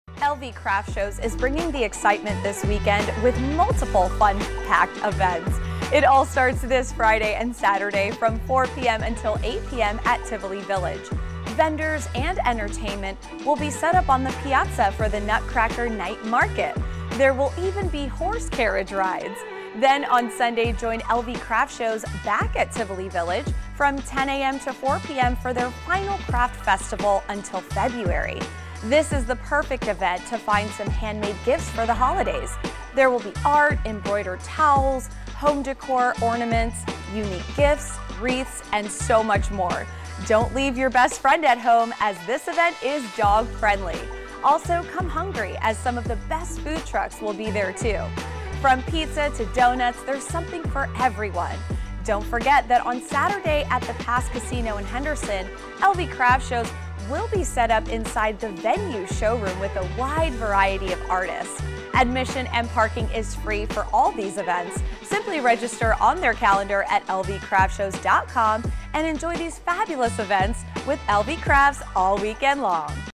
LV Craft Shows to Host Weekend Full of Events
This weekend LV Craft Shows host a Holiday Bazaar in Henderson on Saturday, Craft Festival on Sunday, and Nutcracker Night Market on Saturday and Sunday, both at Tivoli Village.